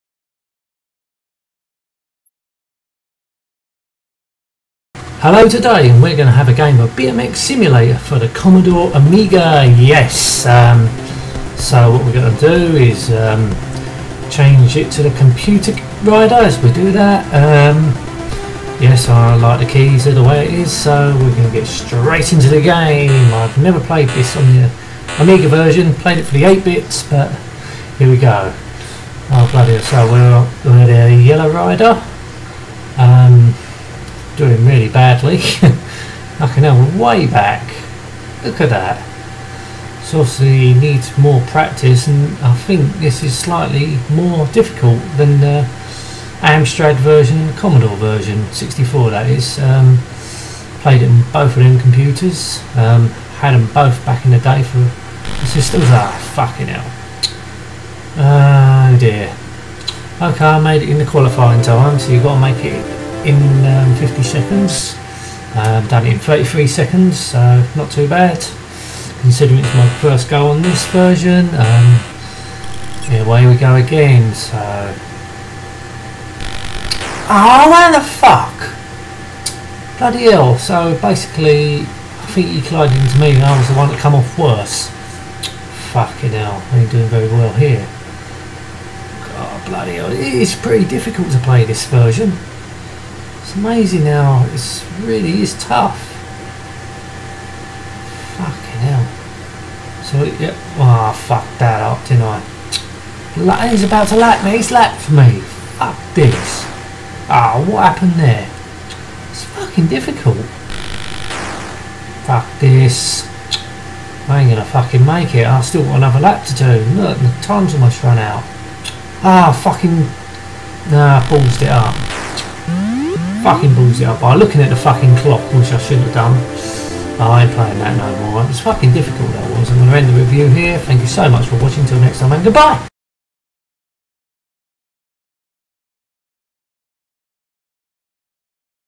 BMX Simulator Review for the Commodore Amiga by John Gage
BMX Simulator is a top down BMX racing game that came out in 1988 by Codemasters. Warning! This video has swearing.